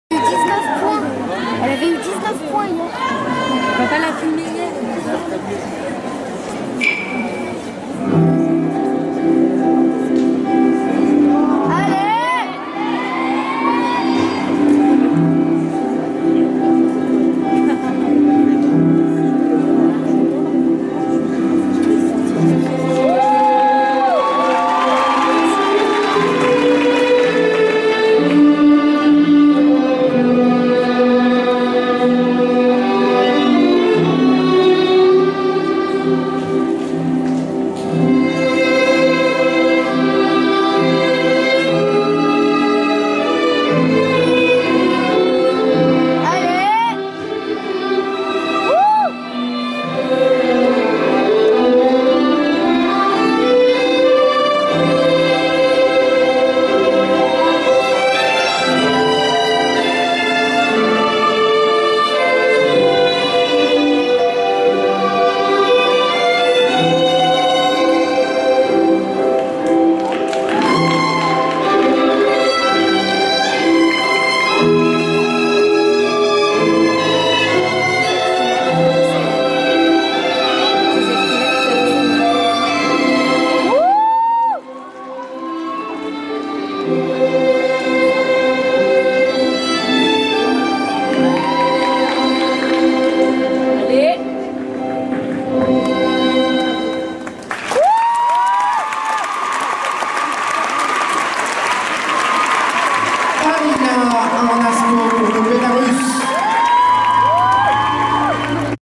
Alina HARNASKO (BLR) ball - 2017 Thiais EF
Alina HARNASKO (BLR)

'17 Thiais Tournament Event Final / Internationaux de Thiais Finales

16.900 (Diff: 8.4 / Ex: 8.5)
Rank : 4th